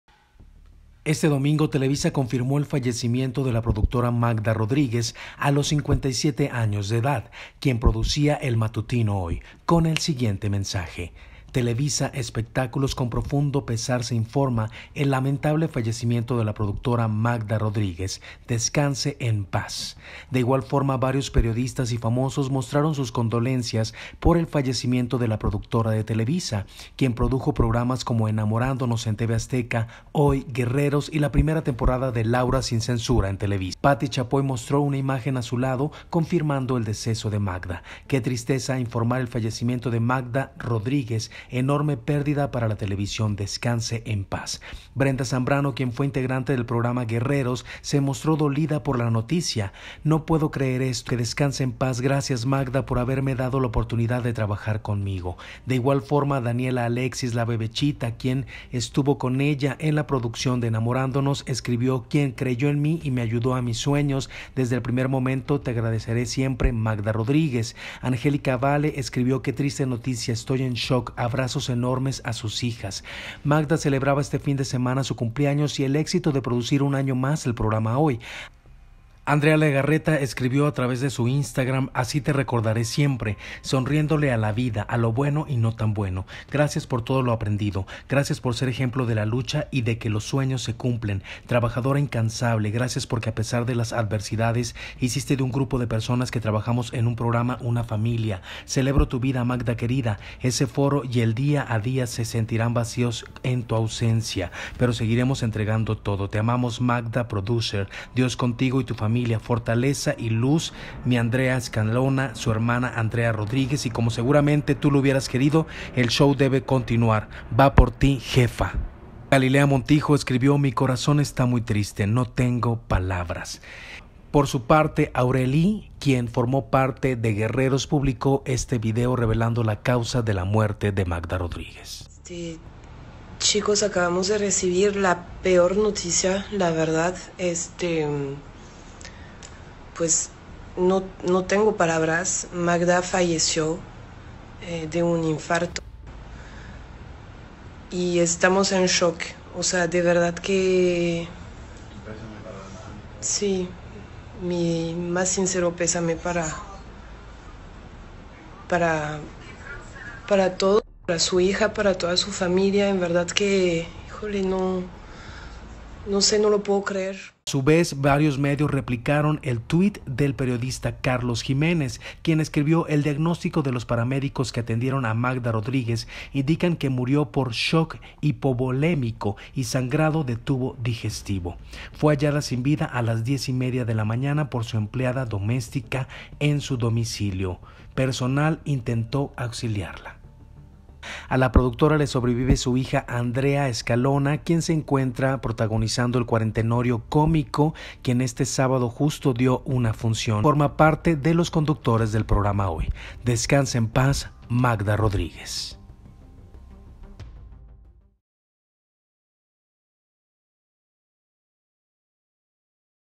Vaya Vaya🤔 : Revelan causa de muerte de Magda Rodríguez
Muere  Magda Rodriguez este domingo 1 de noviembre por la mañana, famosos e influencers se despiden de la productora del programa Hoy. Descanse en paz.